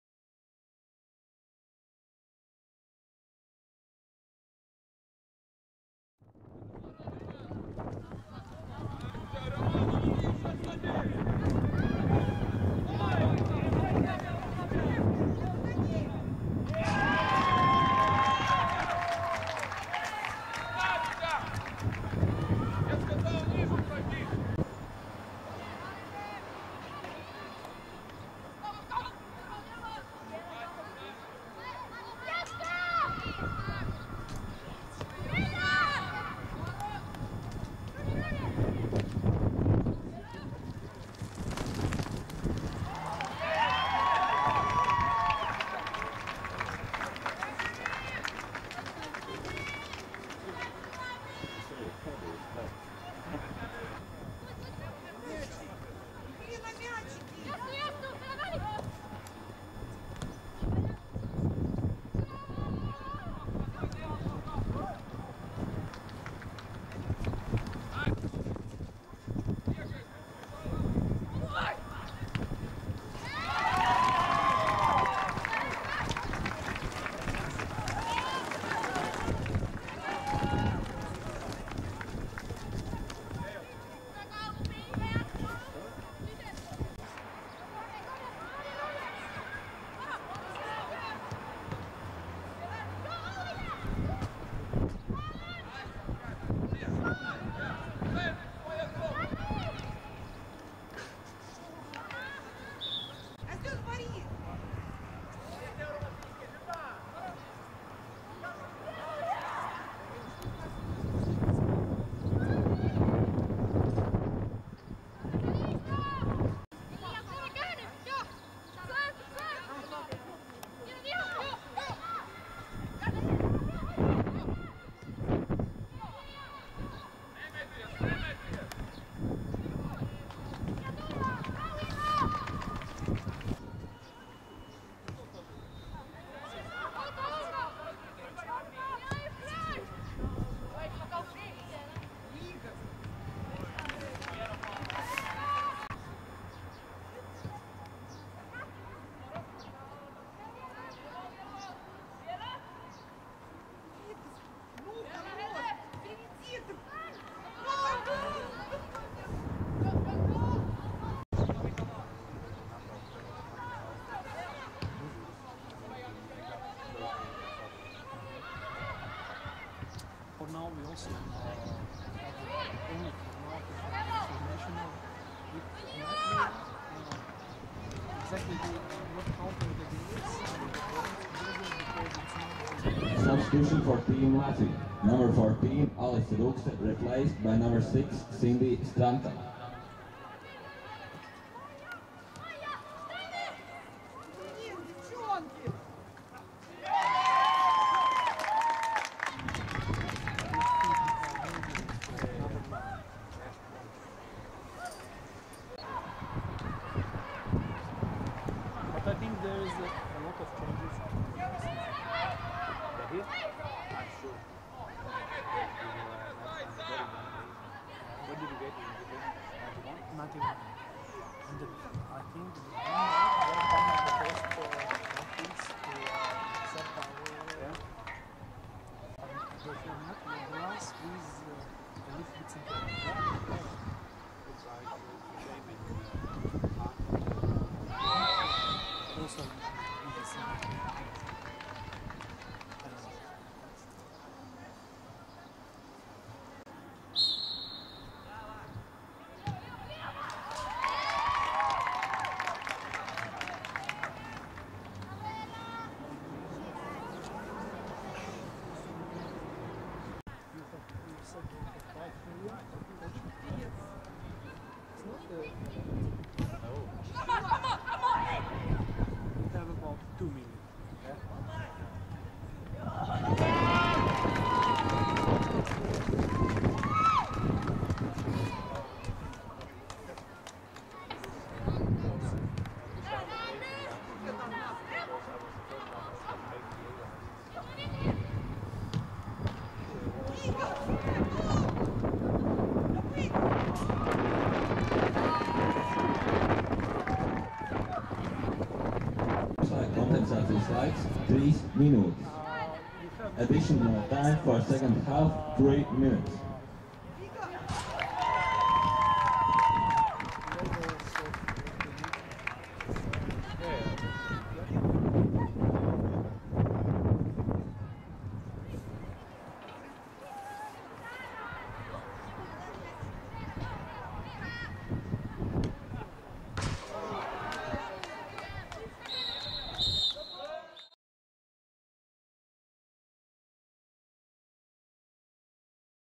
2013.05.05. LATVIJA - SOMIJA (0:9) UEFA Attīstības turnīrs meitenēm
UEFA Attīstības turnīru meitenēm ar zaudējumu Somijas izlasei svētdien, 5.maijā uzsāka Latvijas U-16 meiteņu futbola izlase (1997.gadā dzimušās spēlētājas). Spēles protokols pieejams